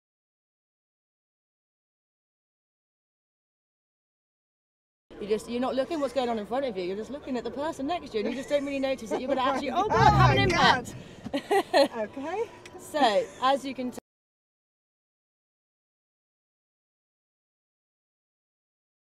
STOPTHECRASH at The London Motor Show - Tracey Wesselingh
Autonomous Emergency Braking has been described by experts as the most significant development in vehicle safety since the seatbelt. The technology works to stopthecrash happening in the first place by automatically applying the brakes in an emergency situation, if the driver doesn’t respond.
We’re at the London Motor Show helping car buyers experience AEB, learn more about tyre safety and ensure the maximum safety benefit on their next purchase by asking 3 life-saving questions about their potential new purchase, namely
What Euro NCAP test rating does this car have?
Is Autonomous Emergency Braking fitted to this car – and is it standard fit?
What else can I do to maximise the safety benefit offered by this car?

Personal Autonomous Emergency Braking (AEB) experience at the STOPTHECRASH stand at the London Motor Show.
To find out more about AEB fitment and performance and to learn more about car safety and technology, please